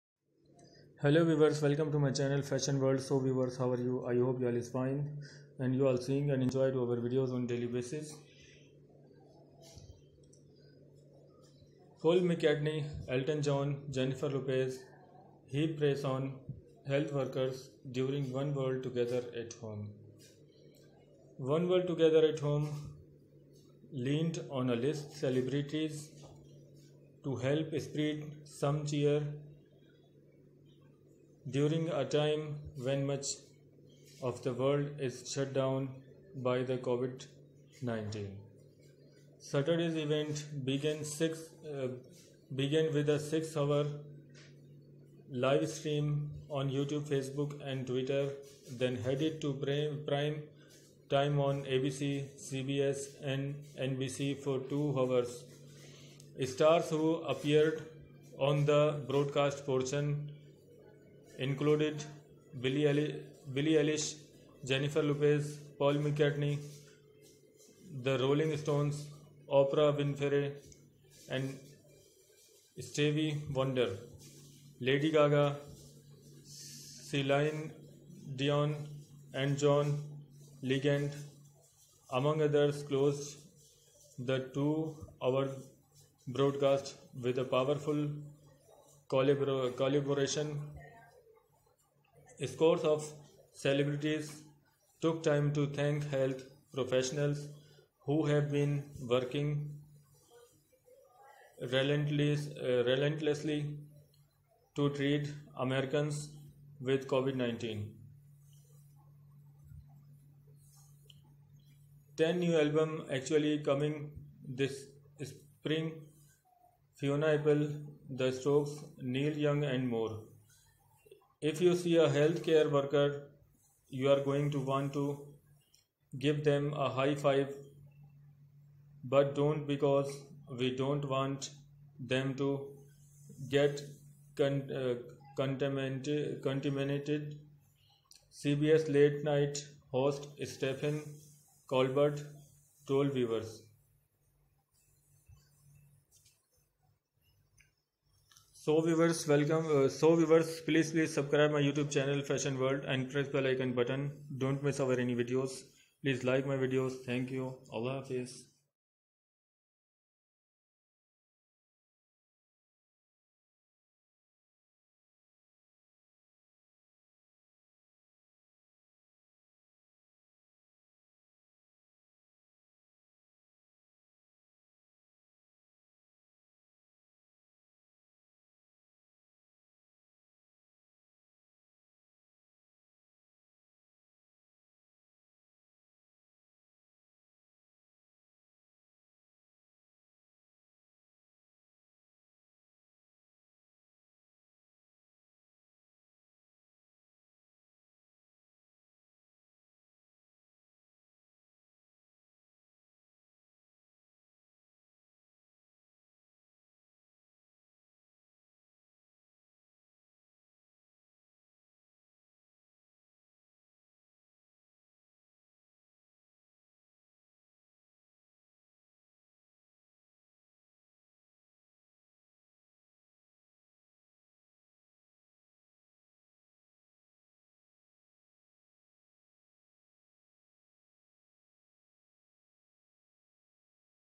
Paul McCartney, Elton John, Jennifer Lopez heap praise on health workers during 'One World:
Saturday's event began with a six-hour livestream on YouTube, Facebook and Twitter, then headed to prime time on ABC, CBS and NBC for two hours. Stars who appeared on the broadcast portion included Billie Eilish, Jennifer Lopez, Paul McCartney, the Rolling Stones, Oprah Winfrey and Stevie Wonder.

Lady Gaga, Celine Dion and John Legend, among others, closed the two-hour broadcast with a powerful collaboration.

Scores of celebrities took time to thank health professionals who have been working relentlessly to treat Americans with COVID-19.